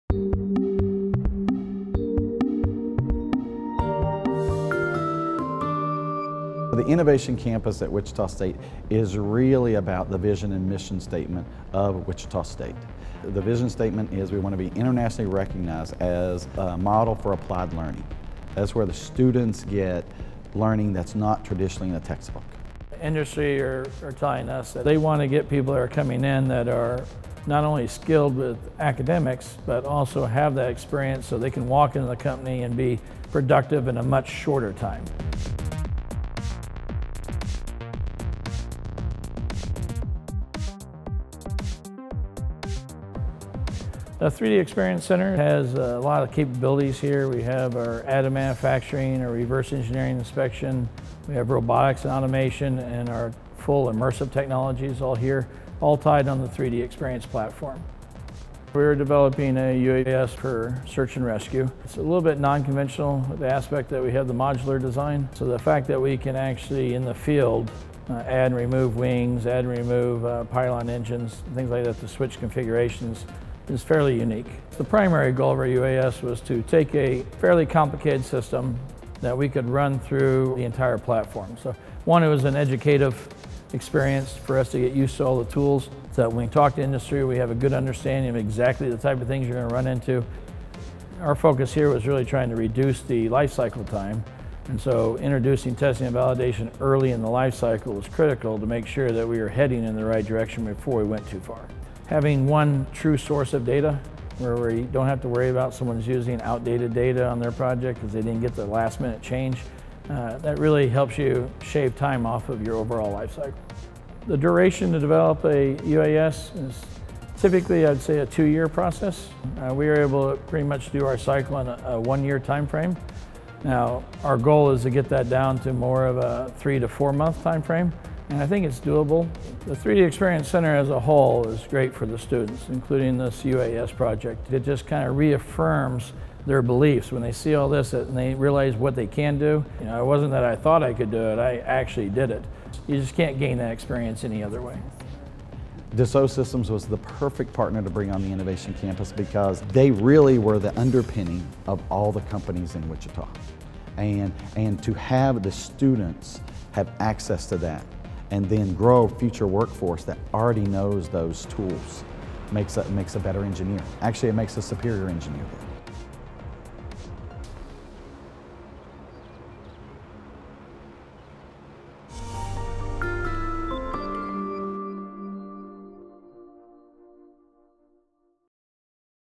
Wichita State University Transforming Applied Learning with 3DEXPERIENCE - Dassault Systèmes
Wichita State University Transforming Applied Learning with 3DEXPERIENCE - Dassault Systèmes. National Institute for Aviation Research (NIAR) at Wichita State University (WSU) partnered with Dassault Systèmes’ to create the first 3DEXPERIENCE Center as part of its newly created Innovation Campus with the 3DEXPERIENCE platform at its heart. Thanks to an integrated platform with unified access to a system digital mockup, composites design technologies, project management, and multi-physics virtual simulation, the student team is working to trim development time of their UAS by 50%.